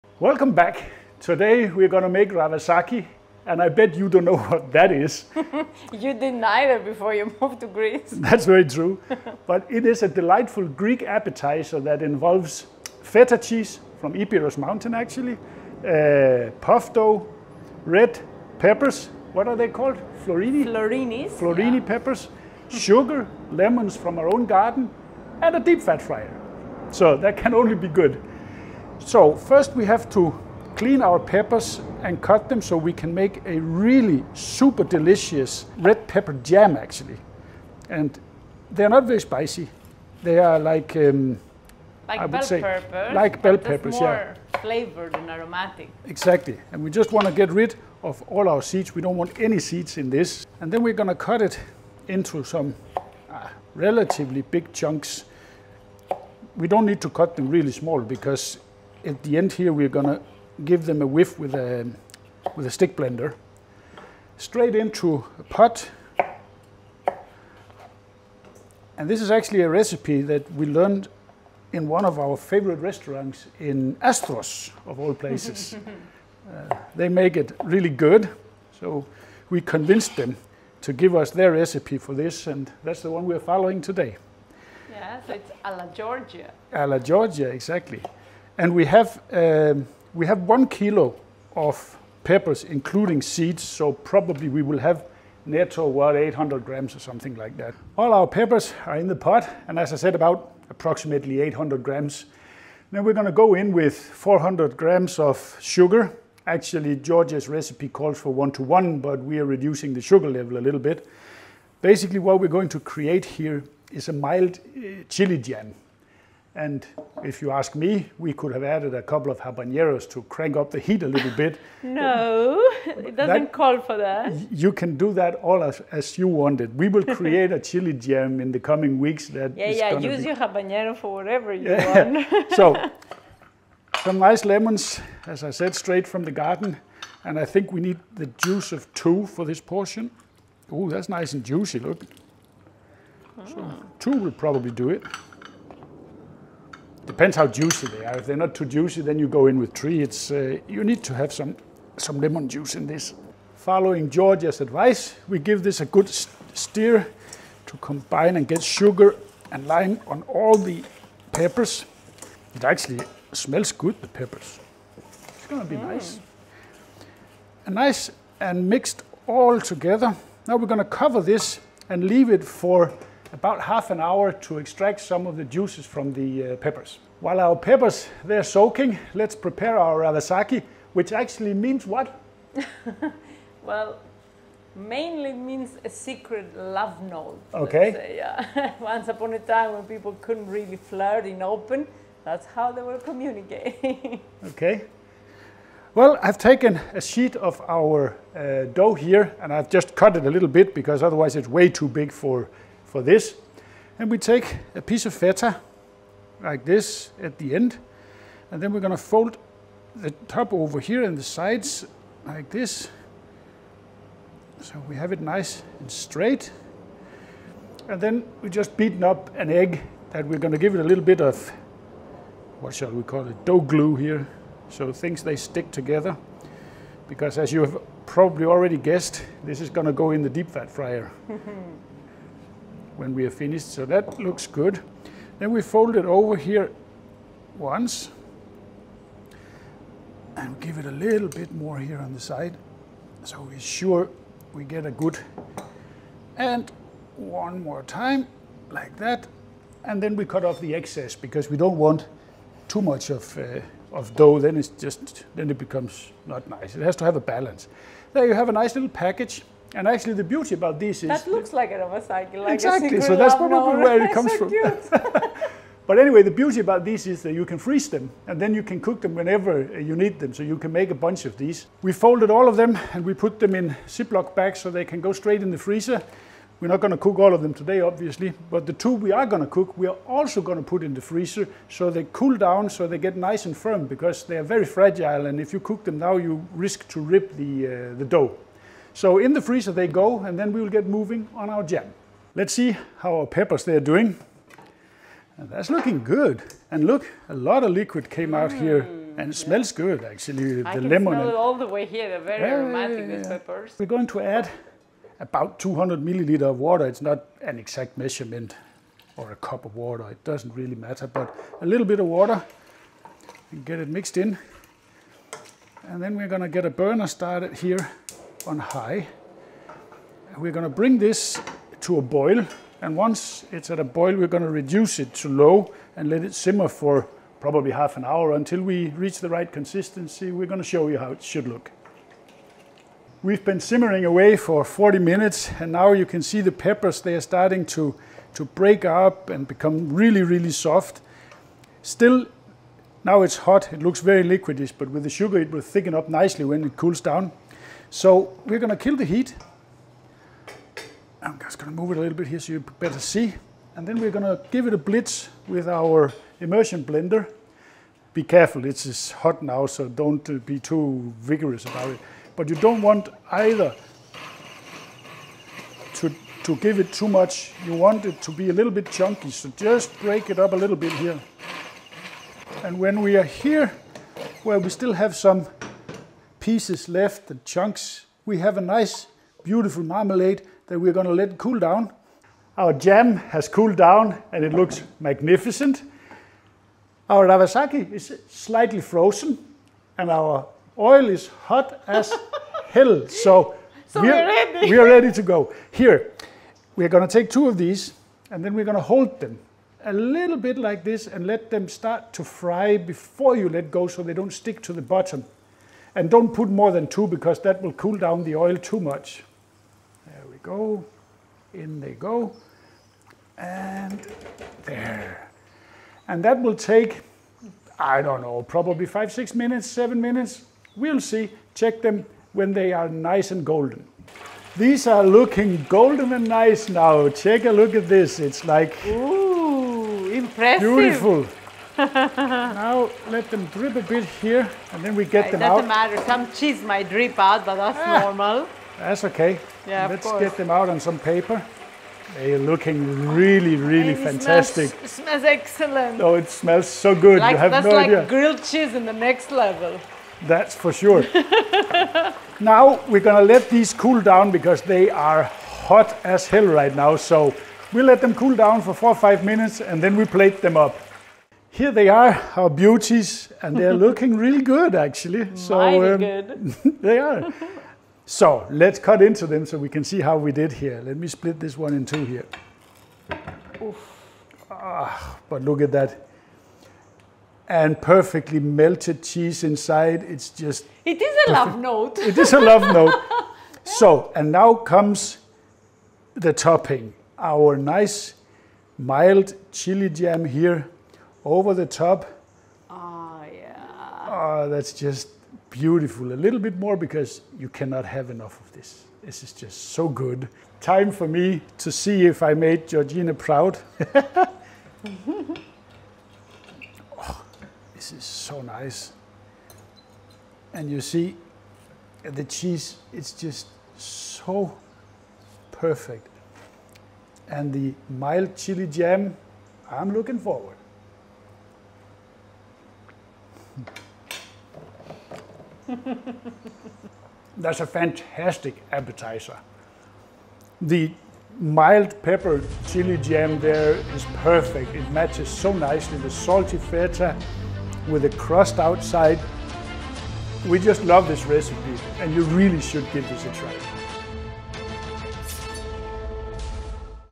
From Greece with Love: Ravasaki & Red Pepper Jam Tutorial
Ravasaki: A Greek Culinary Gem

Indulge in the flavors of Greece with our special recipe for Ravasaki, a delightful snack that translates to "secret love note." This dish perfectly encapsulates the romance and richness of Greek cuisine.

Ingredients:

Puff pastry dough, ready to envelop our star ingredient.
Feta cheese, the quintessence of Greek flavors, crumbly and tangy.
Homemade Red Pepper Jam, adding a sweet and spicy embrace to the dish.
Recipe Journey:

Preparation: We start by cutting the puff pastry into ideal sizes, ensuring each piece is ready to hold its precious feta cargo.
Filling the Love Notes: Each pastry piece is lovingly filled with a generous slice of feta, folding over to create a perfect parcel.
The Golden Dip: These parcels are then deep-fried to a golden perfection, achieving a crispy exterior that contrasts beautifully with the creamy feta inside.
The Sweet Spicy Fusion: Served with our homemade Red Pepper Jam, the Ravasaki transcends into a culinary masterpiece, balancing spicy, sweet, and salty flavors.
Why Ravasaki?

It's a fusion of textures and tastes, offering a crunchy outside with a meltingly soft inside.
The homemade Red Pepper Jam is a game-changer, introducing a sweet and spicy element that complements the salty feta.
Ravasaki is not just food; it's a story of Greek tradition, wrapped in a pastry, a bite-sized piece of cultural heritage.

Join Us: Dive into this recipe with us and discover why Ravasaki is more than just a snack - it's a love note from Greece to your palate.

Ingredients:

Ravasaki:

Feta cheese
Puff pastry
2 eggs

Pepper Jam:

1 Kg / 2 lbs Red Peppers (choose variety depending on how spicy you want it)
400 g / 14 oz Sugar
2 Lemons, juiced
250 ml / 1 cup water